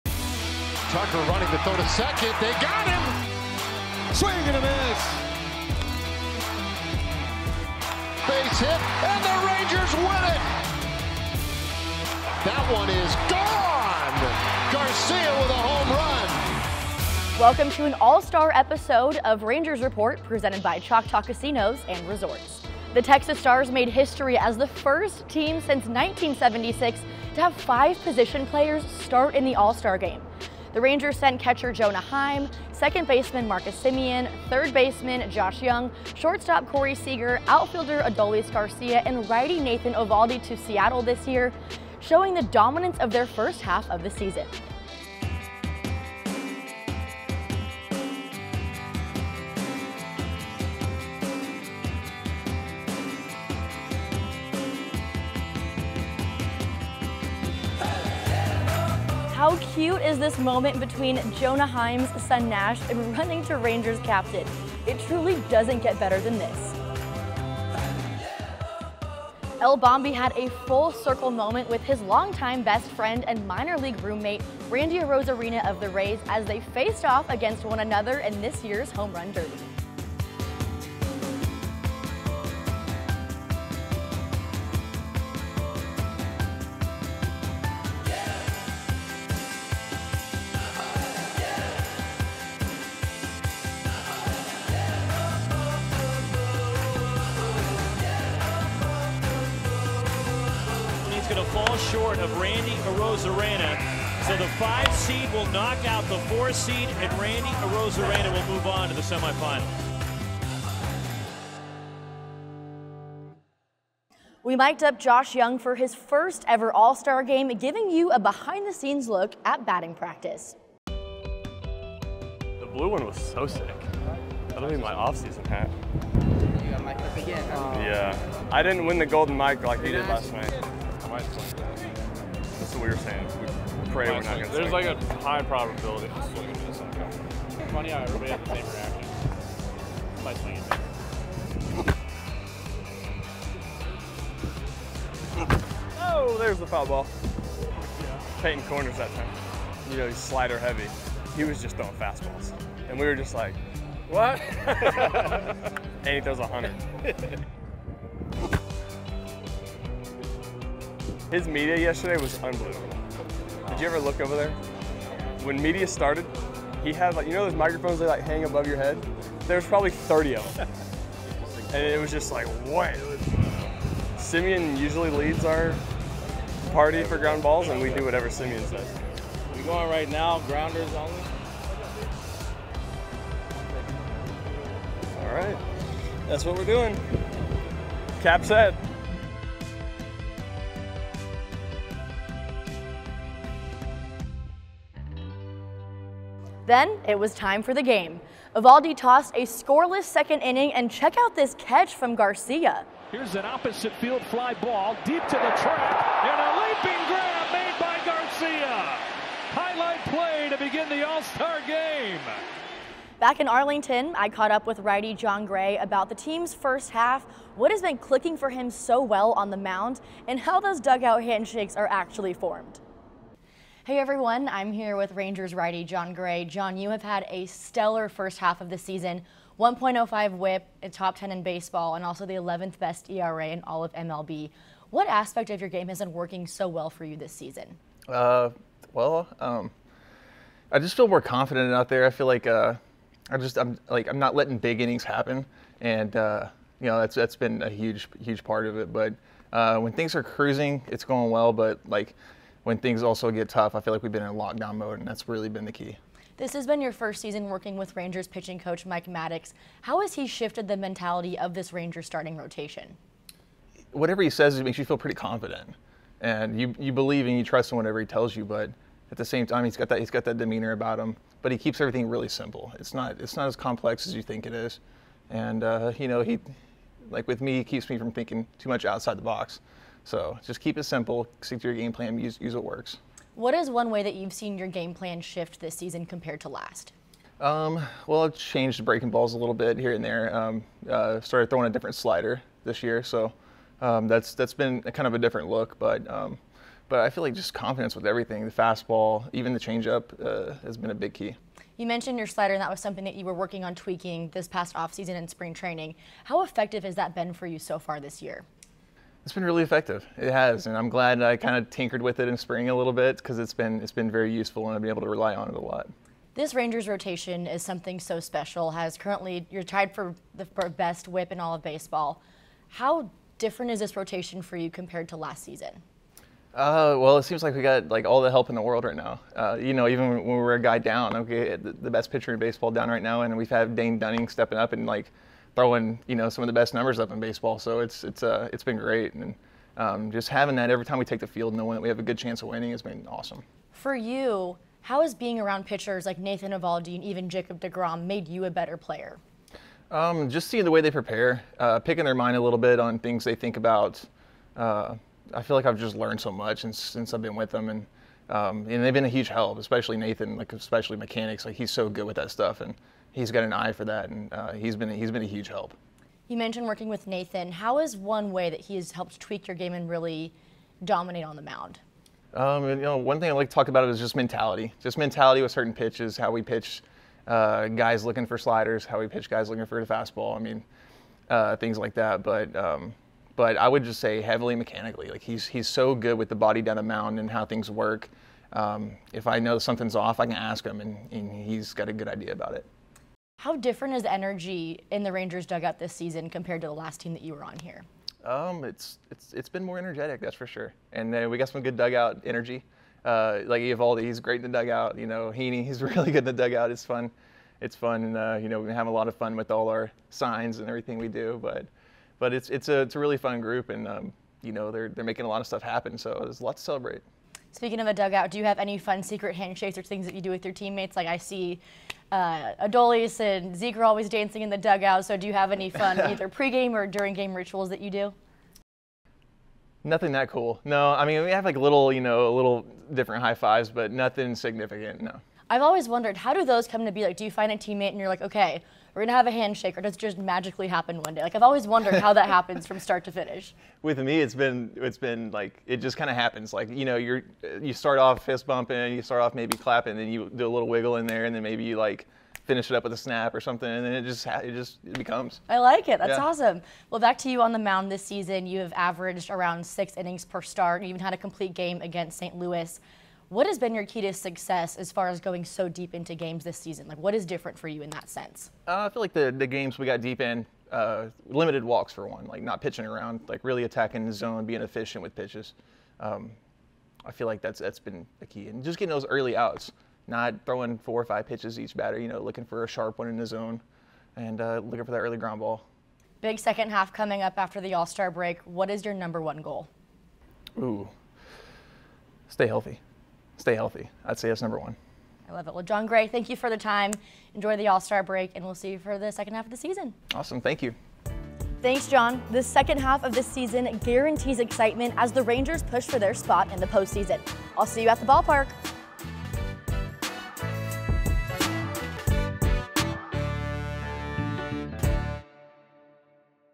Rangers Report - All-Star 2023
Digital Host Hannah Wing recaps the All-Star studded trip to Seattle & sits down with Rangers RHP Jon Gray for a one on one interview.